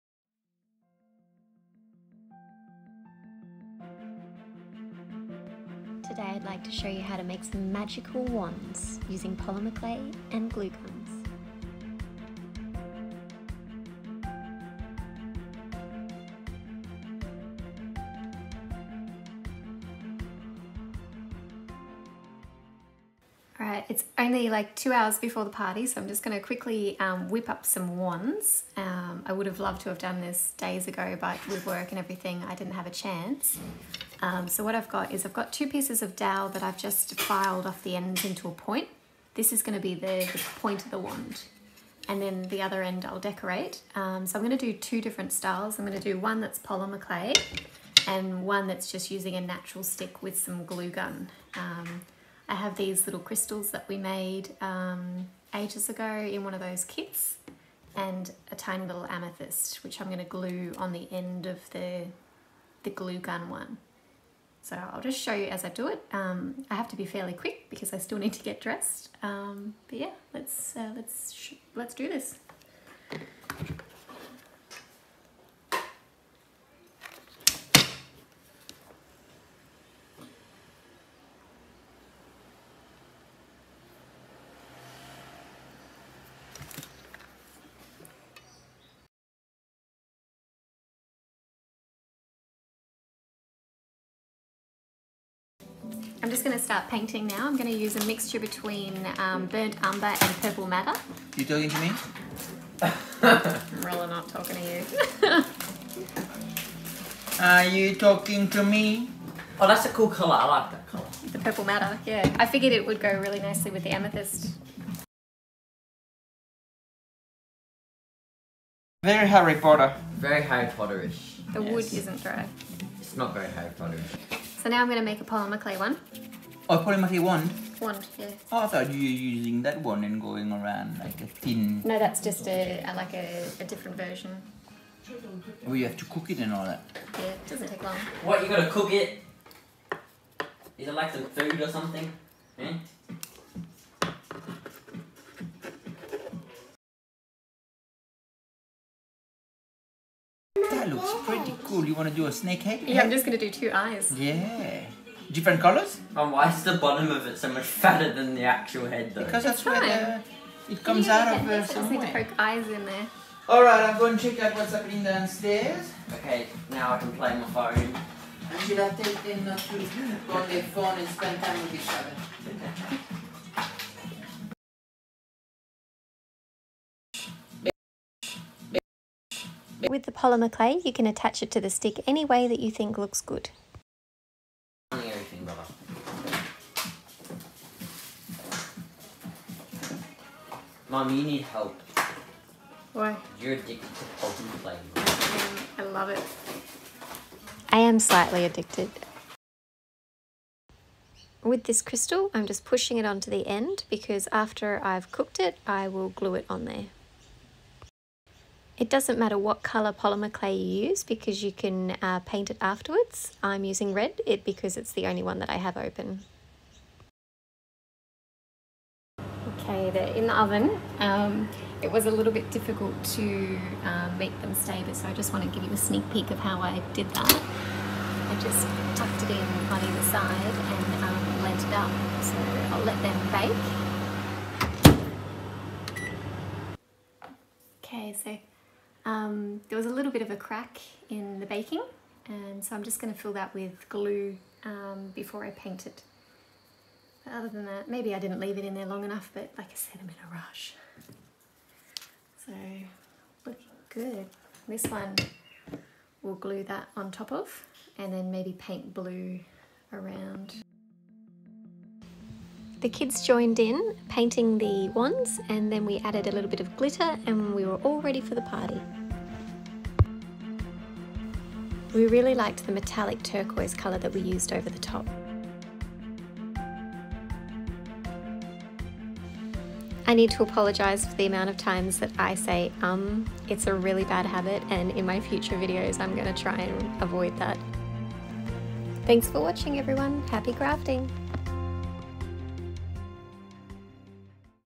Create your own Magical WANDS!
Using a glue gun or polymer clay make your own dream wand!  Harry Potter fans will delight in  this fun and easy project project.